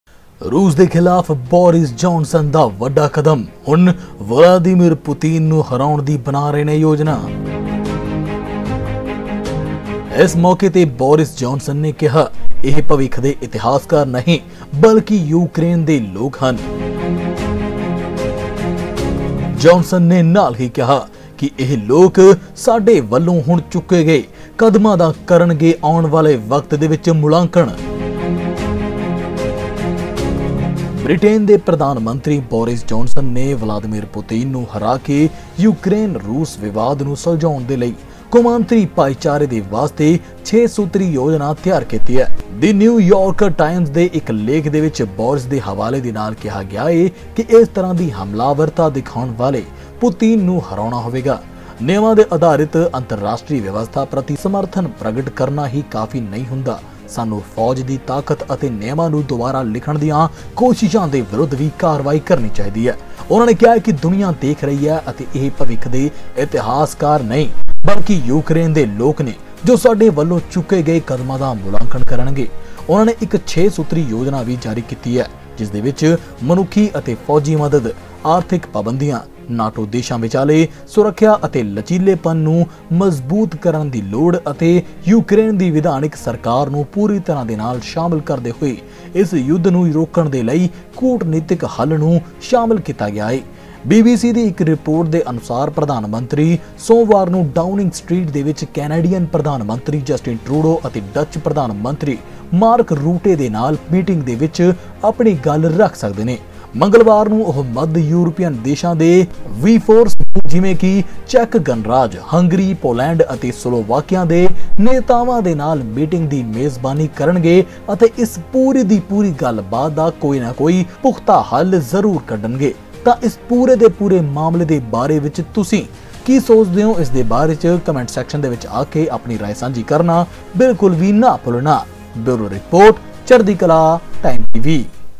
Russia Ukraine War | Boris Johnson ਦੇ ਤੇਵਰ Putin ਖ਼ਿਲਾਫ਼ ਖੋਲਿਆ ਮੋਰਚਾ ...
*  ਇਹ ਭਵਿੱਖ ਦੇ ਇਤਿਹਾਸਕਾਰ ਨਹੀਂ, ਬਲਕਿ ਯੂਕ੍ਰੇਨ ਦੇ ਲੋਕ ਹਨ : ਜਾਨਸਨ
*  ਜੋ ਸਾਡੇ ਵੱਲੋਂ ਹੁਣ ਚੁੱਕੇ ਗਏ ਕਦਮਾਂ ਦਾ ਮੁਲਾਂਕਣ ਕਰਨਗੇ : ਜਾਨਸਨ

ਬ੍ਰਿਟੇਨ ਦੇ ਪ੍ਰਧਾਨ ਮੰਤਰੀ ਬੋਰਿਸ ਜਾਨਸਨ ਨੇ ਪੁਤਿਨ ਨੂੰ ਹਰਾ ਕੇ ਯੂਕ੍ਰੇਨ-ਰੂਸ ਵਿਵਾਦ ਨੂੰ ਸੁਲਝਾਉਣ ਲਈ ਕੌਮਾਂਤਰੀ ਭਾਈਚਾਰੇ ਲਈ ਛੇ-ਸੂਤਰੀ ਯੋਜਨਾ ਤਿਆਰ ਕੀਤੀ ਹੈ। ਦਿ ਨਿਊ ਯਾਰਕਰ ਟਾਈਮਜ਼ ਦੇ ਇੱਕ ਲੇਖ ਵਿਚ ਬੋਰਿਸ ਦੇ ਹਵਾਲੇ ਨਾਲ ਕਿਹਾ ਗਿਆ ਕਿ ਇਸ ਤਰ੍ਹਾਂ ਦੀ ਹਮਲਾਵਰਤਾ ਦਿਖਾਉਣ ਵਾਲੇ ਪੁਤਿਨ ਨੂੰ ਹਰਾਉਣਾ ਹੋਵੇਗਾ।